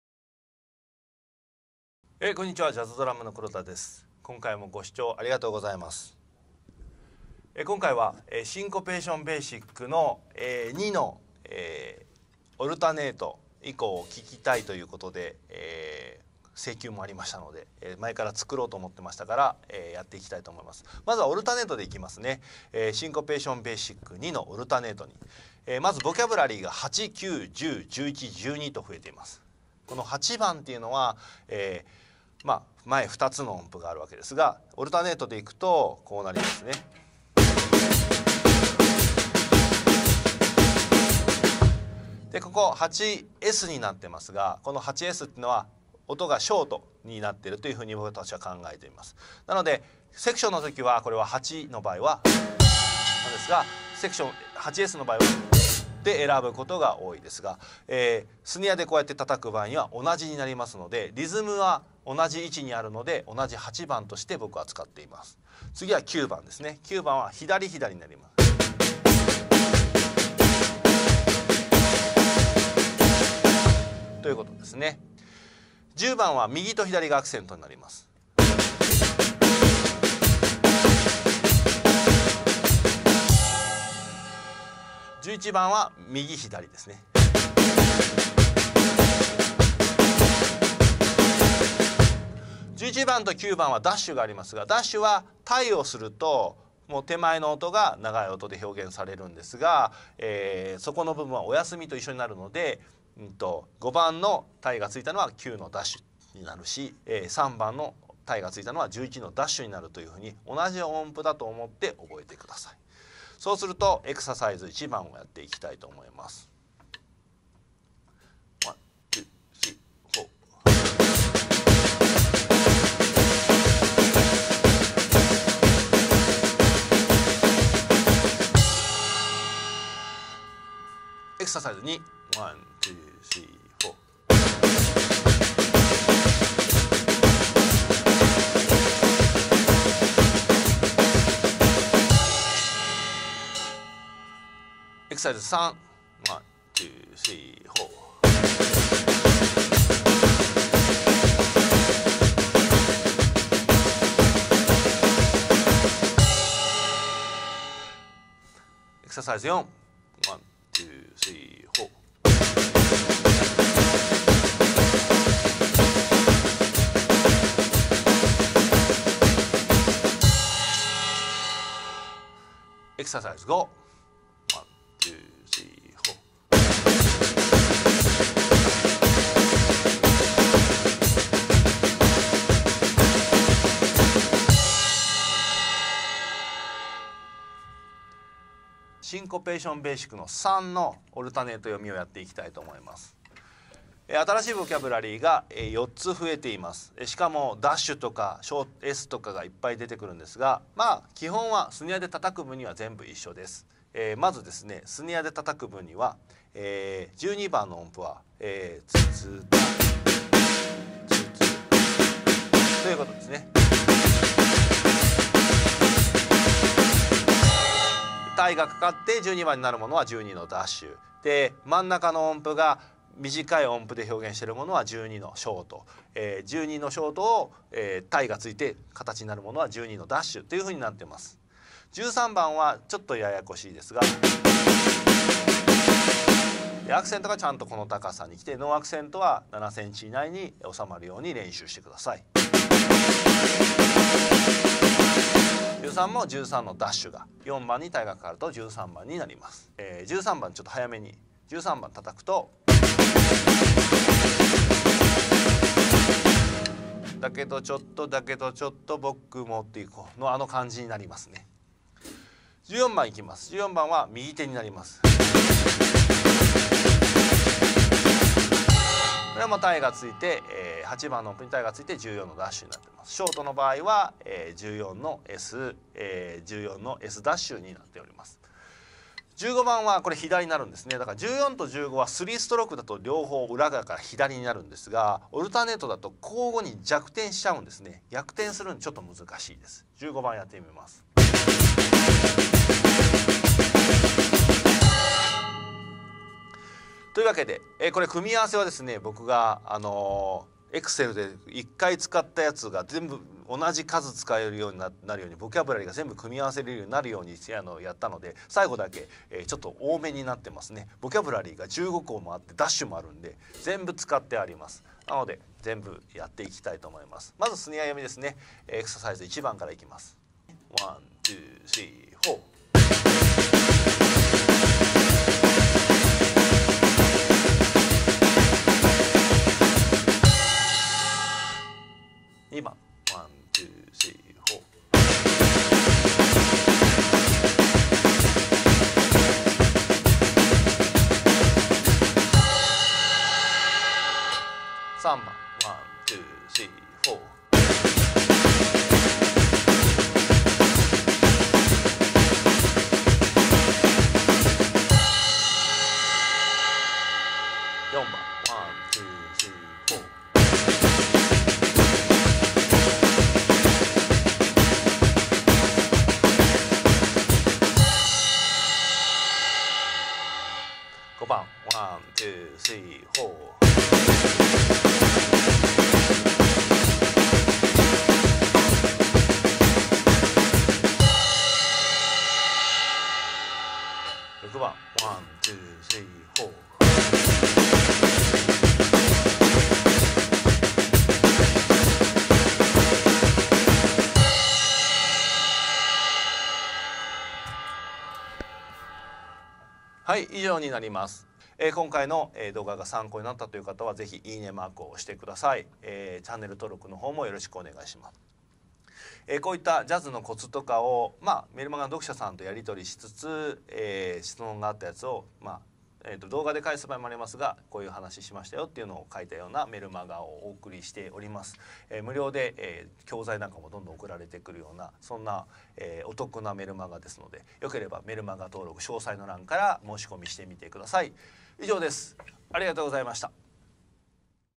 【Syncopation Basic】左右交互altanate 2&3 How to practice for drum solos. How to use the syncopation book.
シンコペーションブックを読めるようにするために
僕独自の譜面での練習方法、第二弾です。
左右交互のアクセント移動の練習方法です。
How to read the syncopation book.
I made a chart to become to read the syncopation book.
This is altanete type.
This video explains how to use it.

無料メールマガジンでは
実際の曲での解説をしています。
音源や譜面もダウンロードできますので